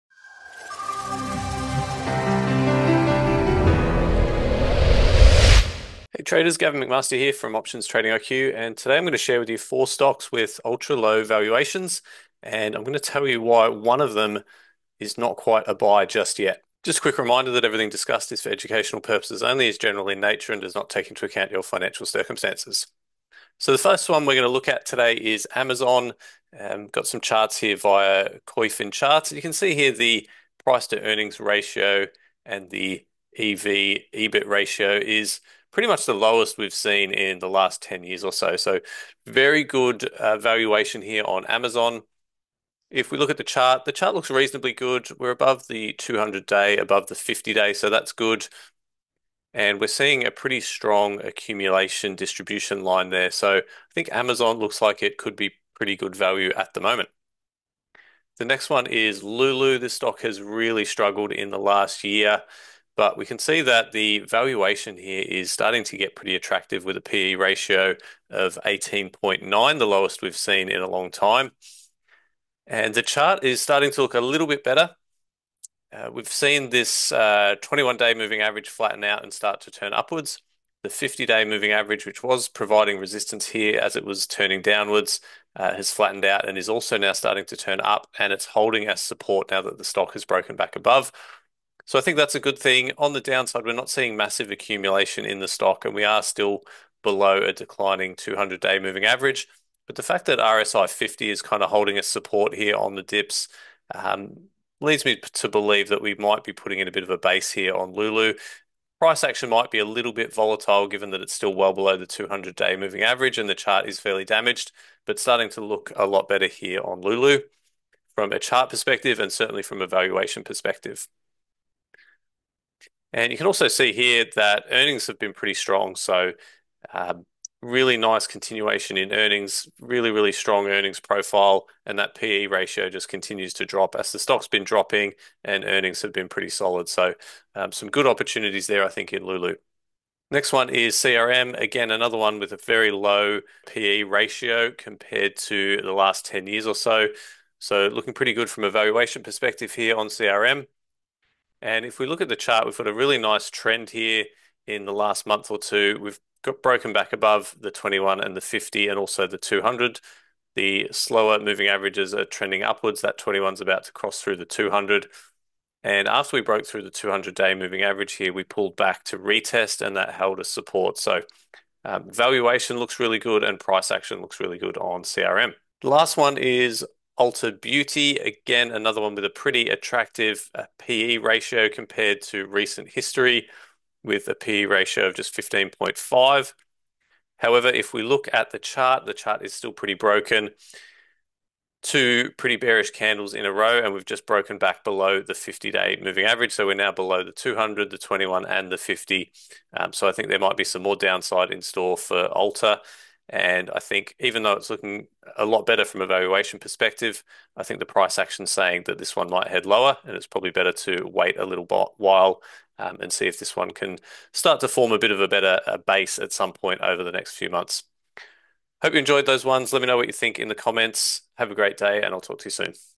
4 Stocks With Ultra Low Valuations
In today’s video, we’re highlighting 4 Stocks with Ultra Low Valuations—Amazon (AMZN), Lululemon (LULU), Salesforce (CRM), and Ulta Beauty (ULTA).

These high-growth companies are currently trading at attractive valuations, offering unique opportunities for investors.

However. one of them is pretty broken in terms of price action and in my opinion, is not yet a buy.